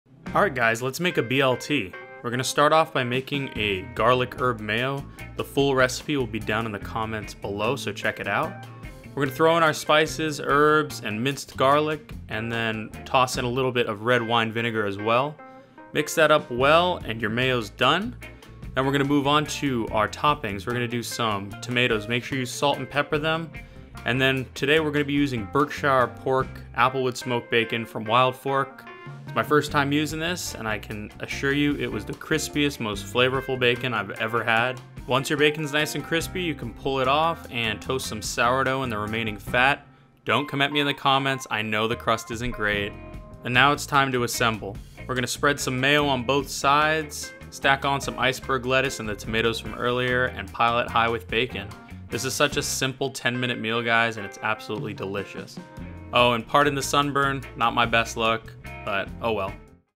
This is The BEST BLT I've Ever Eaten!!!
Today we're making a simple yet delicious meal that takes less than 10 minutes. We are using high quality ingredients across the board, from our garlic herb mayo and Berkshire pork to our rustic bread and fresh vegetables. After you've tried this, you'll never eat a normal BLT again.

Ingredients:

Garlic Herb Mayo:
Mayo
Chili Powder
Dried Oregano
Fresh Thyme
Minced Garlic

Sandwich:
Berkshire Applewood Smoked Bacon
Iceberg Lettuce
Beefsteak Tomatoes
Sourdough Bread